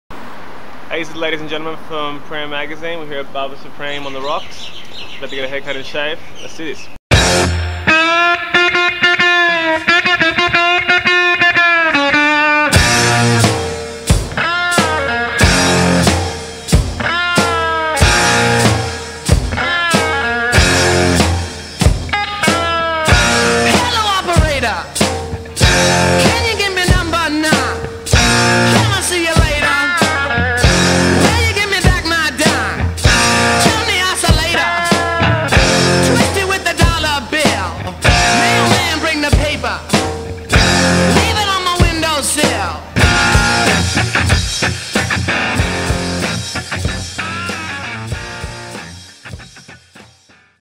Barber Supreme x PRIMO! Magazine
At Barber Supreme the old world charm of the Barber Shop is reimagined for the modern gentleman. This classic cultural experience is provided in a relaxed professional atmosphere with superior tools and products.

PRIMO! magazine is cooler than rocket ships, and high-fives.